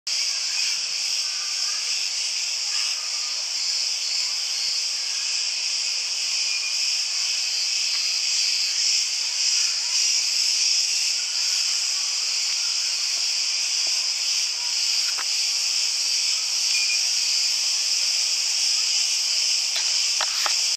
3D цветы и цигады в Херсониссос на острове Крит Греция
3D  цветы и цигады в Херсониссос на острове Крит Греция
3D видео без очков